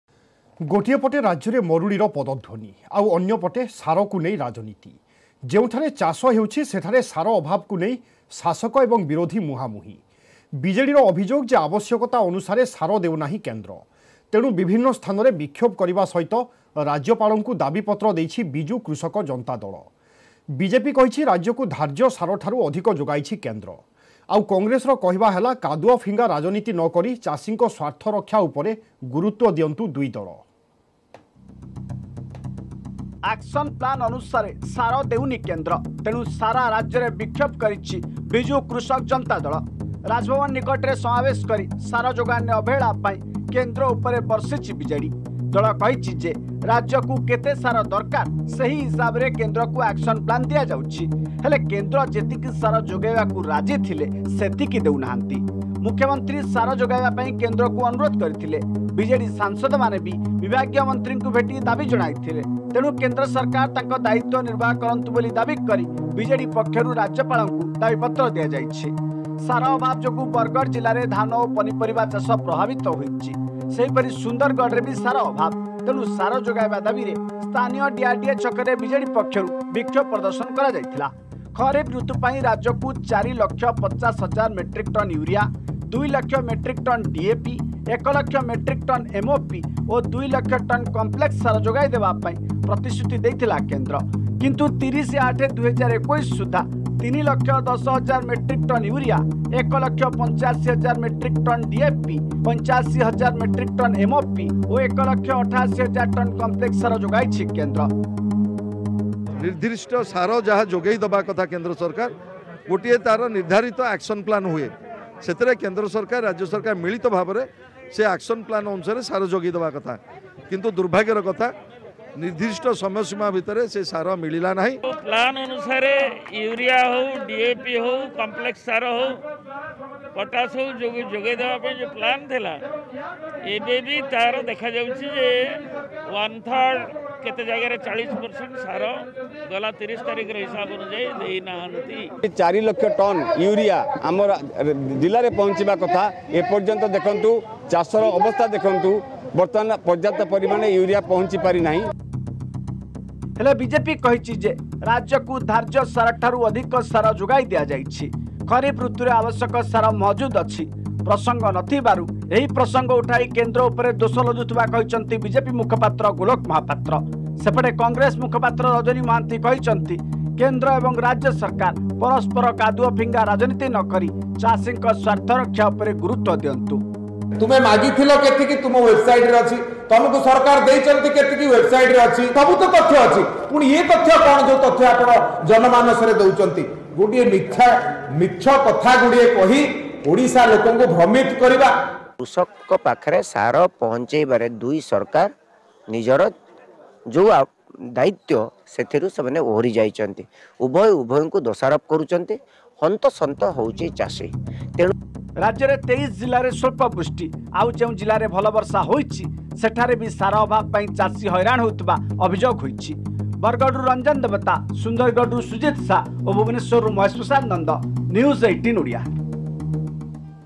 ସାର ଅଭାବକୁ ନେଇ ଶାସକ ଓ ବିରୋଧୀ ମୁହାଁମୁହିଁ, କେନ୍ଦ୍ର ପର୍ଯ୍ୟାପ୍ତ ସାର ଦେଉନଥିବା BJDର ଅଭିଯୋଗ
ରାଜ୍ୟରେ ମରୁଡି ଆଶଙ୍କା ରହିଥିବା ବେଳେ ସାରକୁ ନେଇ ରାଜନୀତି | ସାର ଅଭାବକୁ ନେଇ ମୁହାଁମୁହିଁ ହୋଇଛନ୍ତି ଶାସକ ଓ ବିରୋଧୀ | କେନ୍ଦ୍ର ପର୍ଯ୍ୟାପ୍ତ ସାର ଦେଉନଥିବା ଅଭିଯୋଗ କରିଛି  BJD | 1st Sept 2021